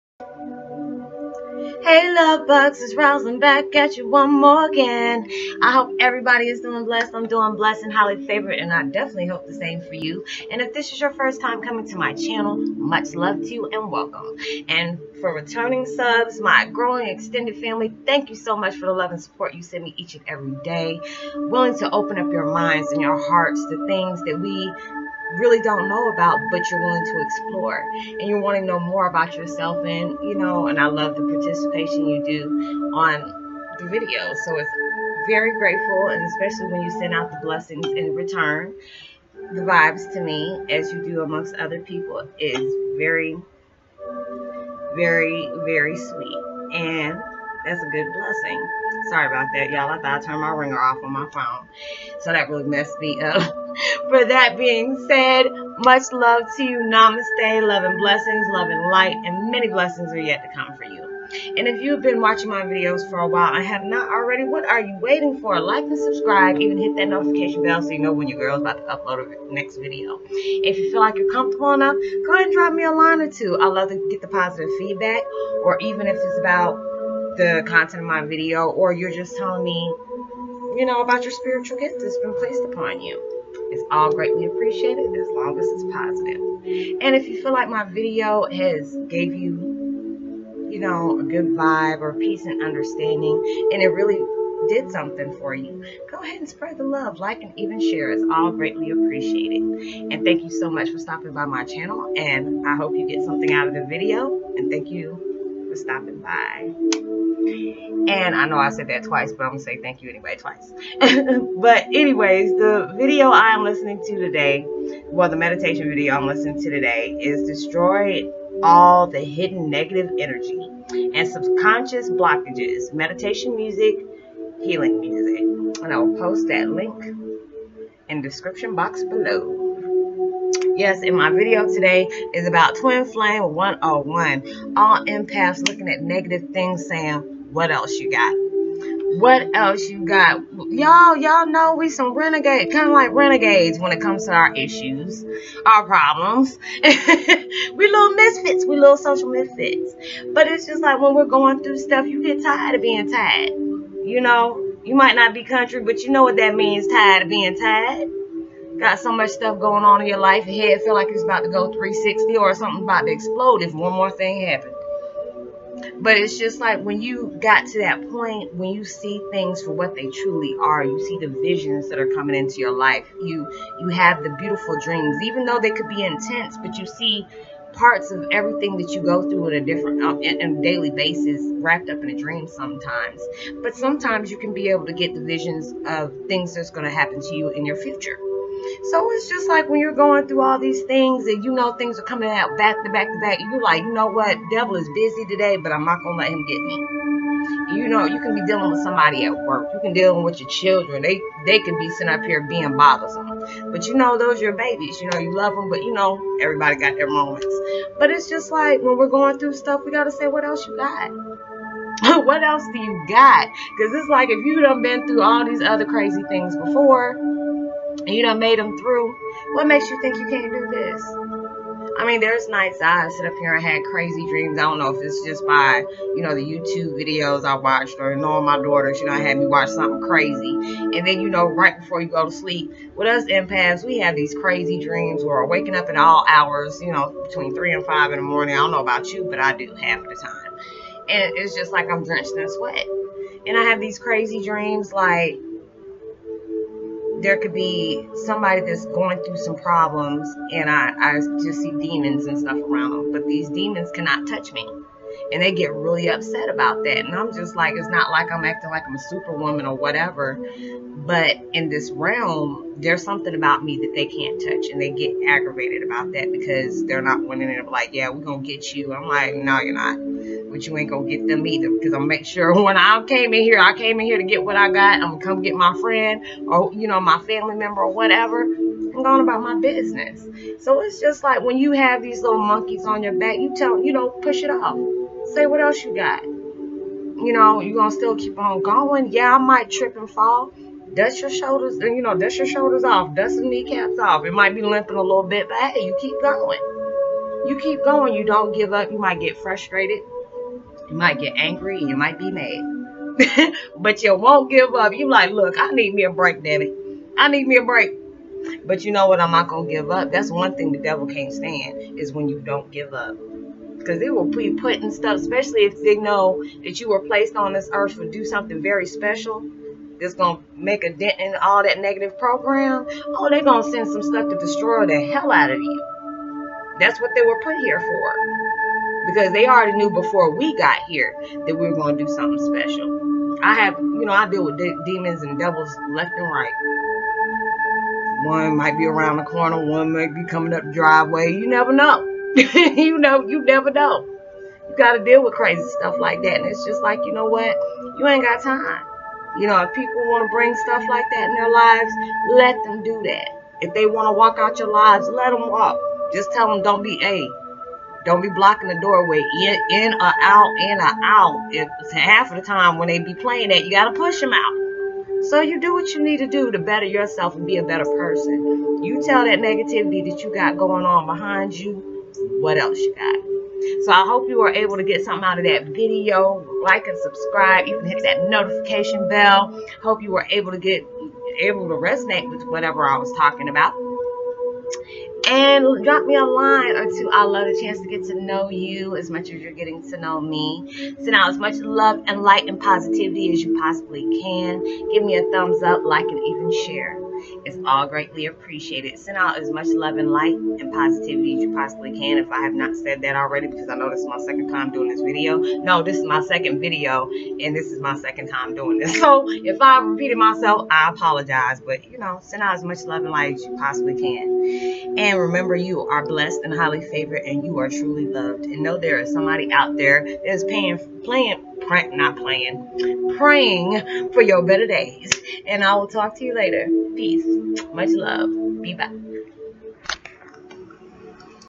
All Empaths Tellin Negativity Wat Else U Got!!!
When your just tired of being tired especially dealing with the negative monkey on your back knowing you've dealt with many troubled situations before and got thru it. Deep down inside you know there's nothing can't thru, you know you were made for this!!!